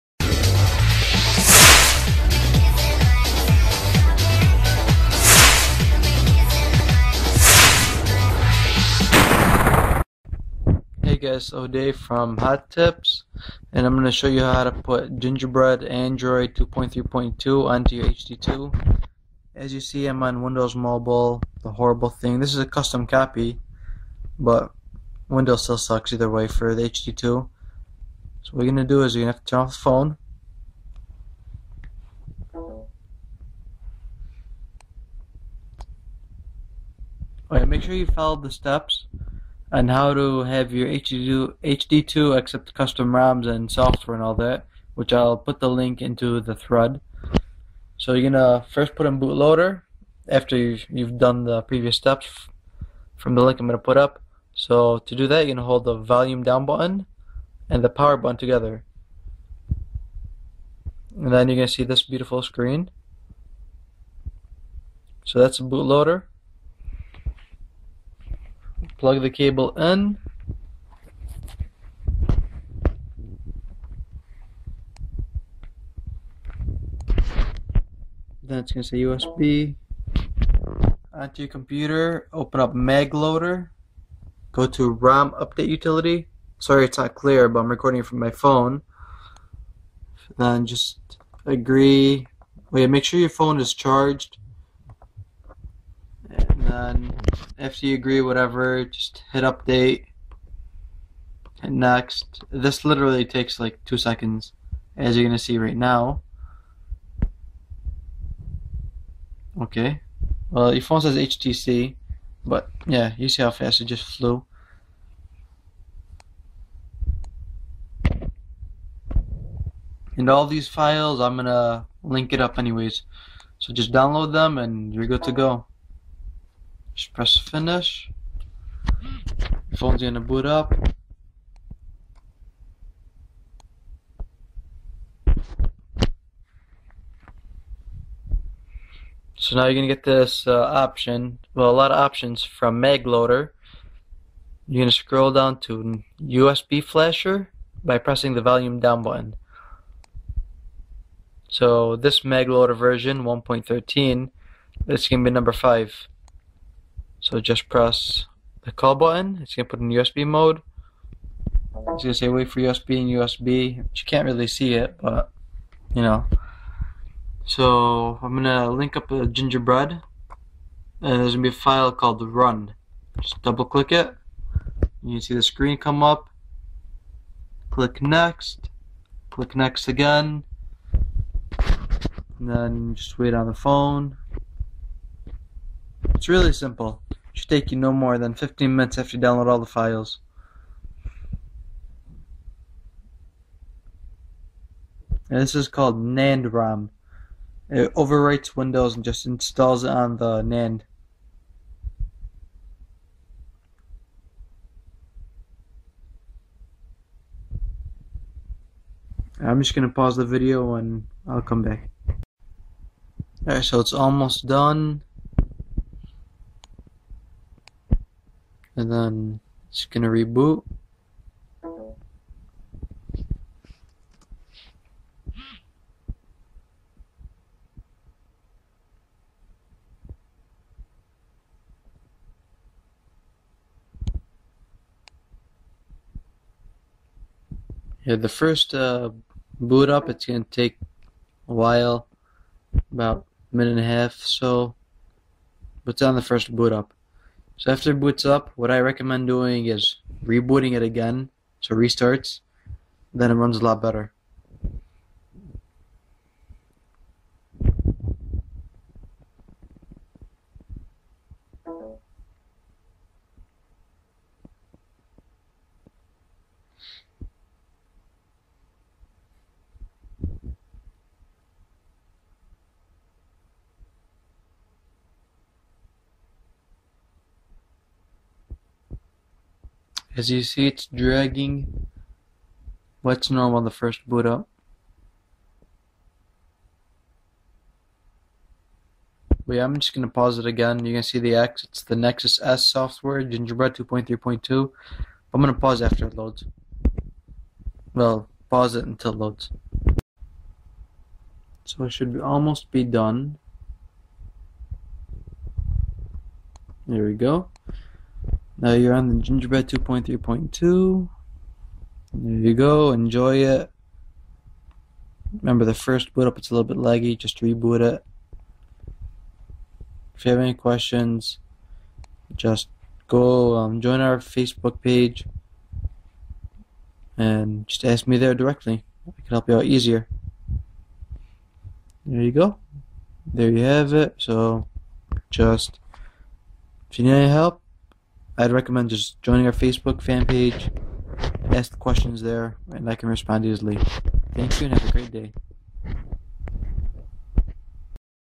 How To Install Android Gingerbread 2.3.2 on the T-Mobile HTC HD2
Install Android Gingerbread over Windows Mobile. Officially boot Android instead!